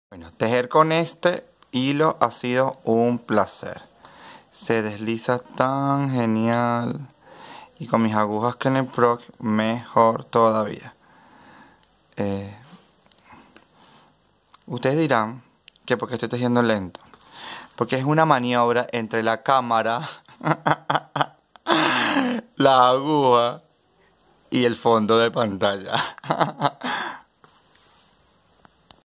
Jugando con mis nuevas agujas Knitpro e hilo "Fidelio"
Un poco de mi cuento de tejer con placer por las agujas Knitpro nuevas y por el hilo "Fidelio"